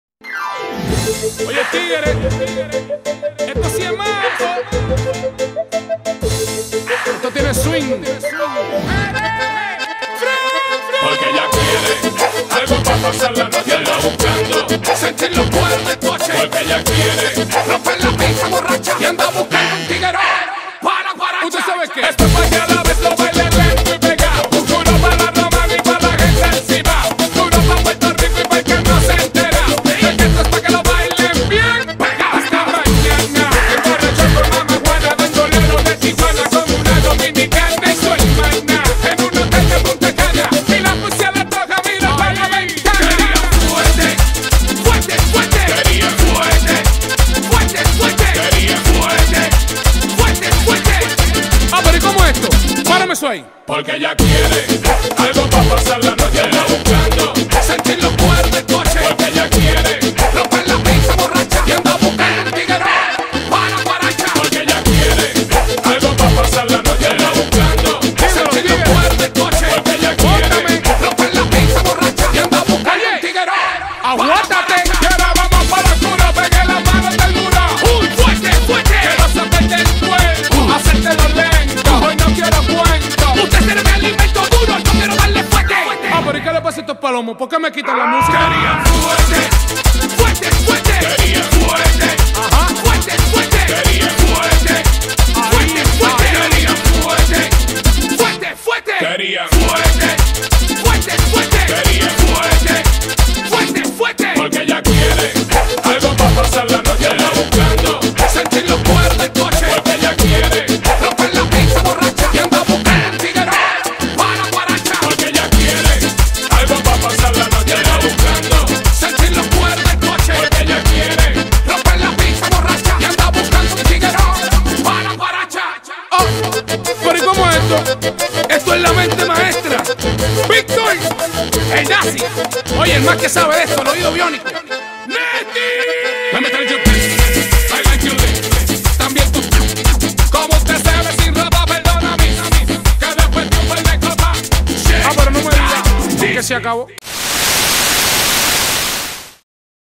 Aprende a bailar
el mejor video de baile donde puedes aprender a bailar reggaeton con dos profesores con experiencia